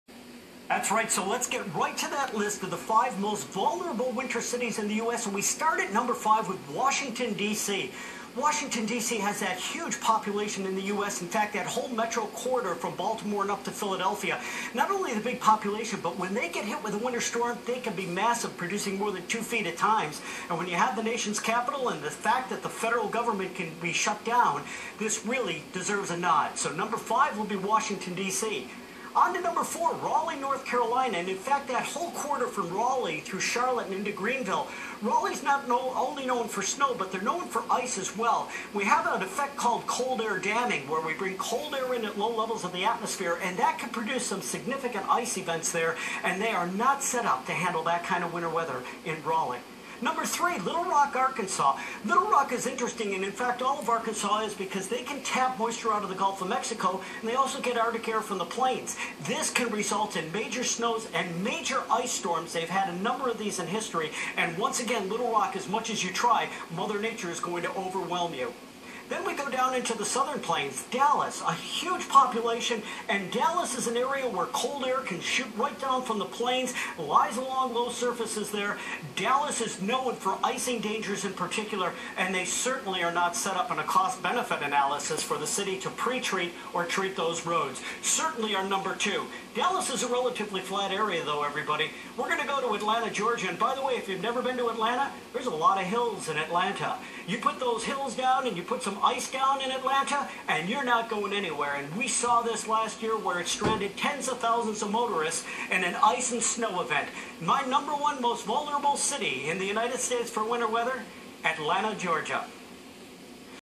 Tom Niziol Top 5 Winter Storms
With a Winter Weather Expert Tom Niziol countdown to A Top 5 Cities about Winter Storms.